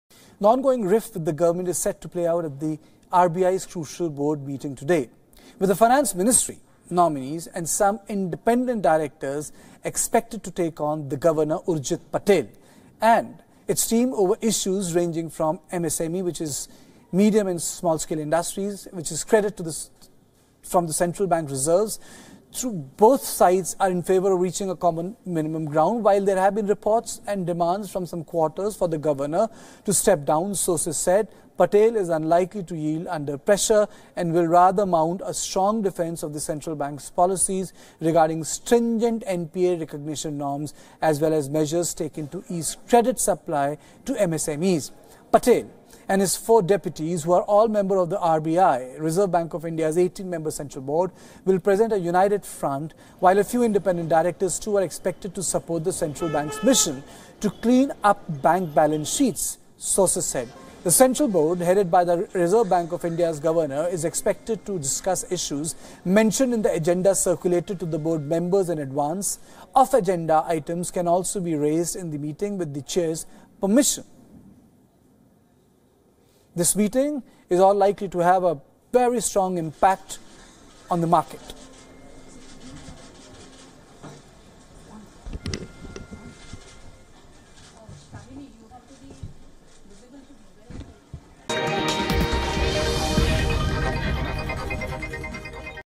RBI, govt crucial meet today
RBI Board meeting will take place today in Mumbai to resolve differences over critical regulatory and bridge the widening rift between the Central Bank and Govt.